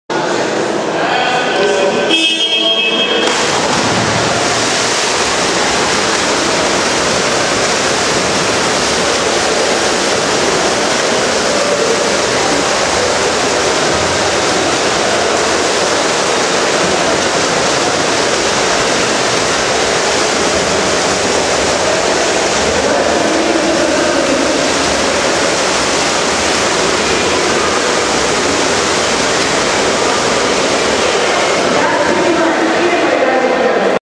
swim_22.MOD
Первенство ВУЗов г.Москвы по летнему полиатлону
19-21 апреля 2012 г., МГУ
Плавание 50 м
Заплыв № 22